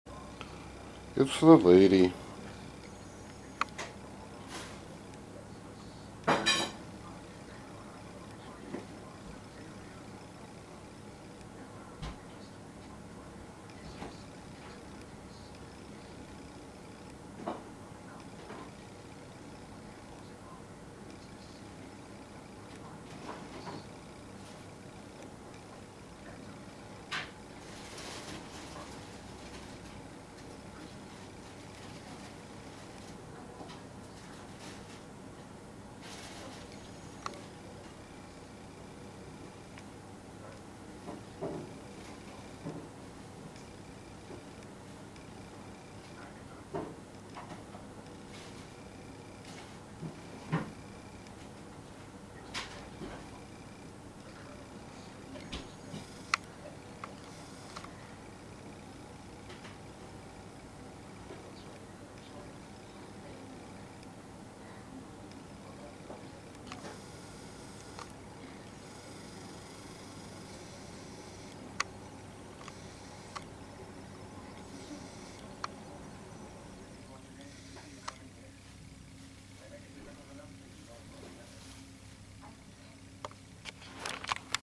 4/24/2015 Lady Crow at the stump
filming Lady Crow from the front window